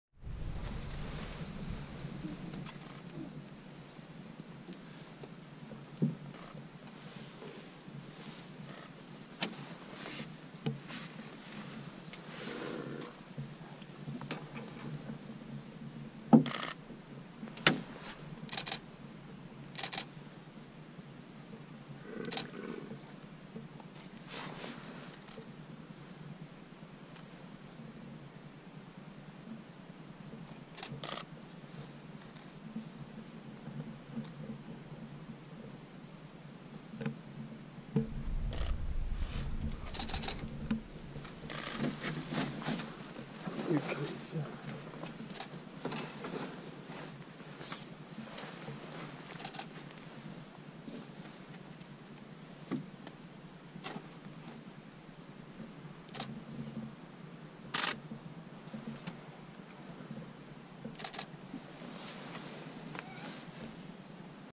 In the boat Lake Kerkini
A video of how you can work from a boat in Lake Kerkini with your Pelican Photography